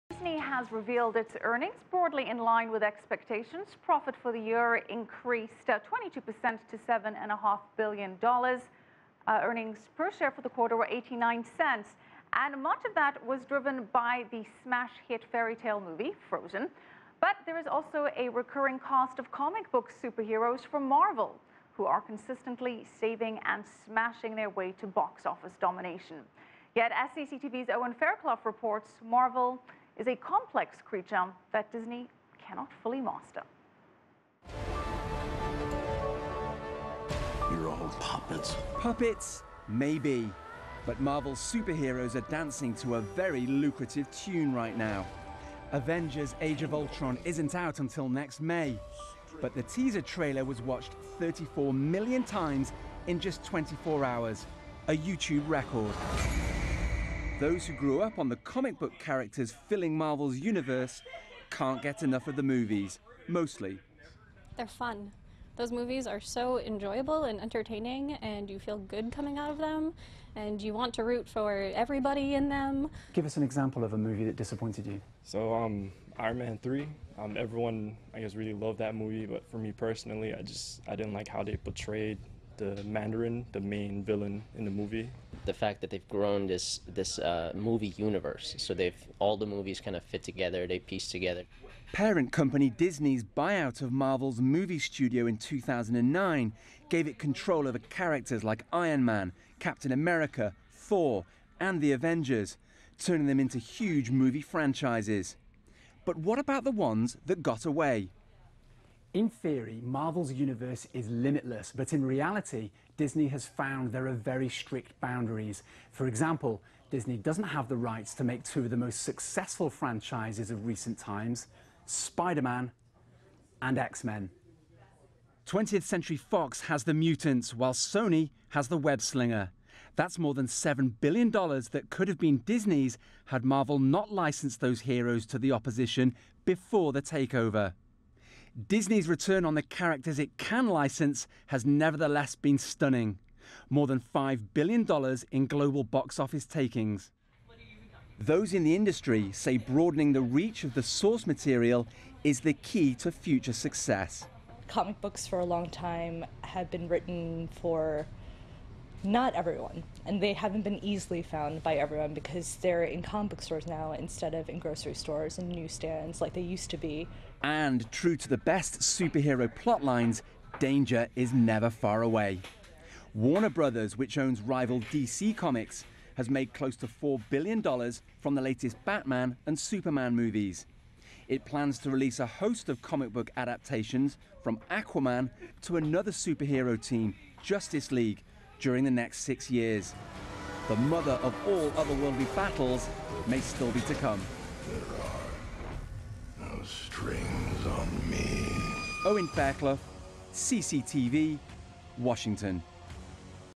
Marvel Comics movies make big money for parent company Disney
Walt Disney has unveiled its best financial results ever. Profits for the year increased 22 percent to $7.5 billion. Its acquisition of Marvel Studios has generated box office gold with movies like Captain America, Thor and The Avengers. CCTV America's Owen Fairclough reported on how Marvel's superheroes are dancing to a very lucrative tune.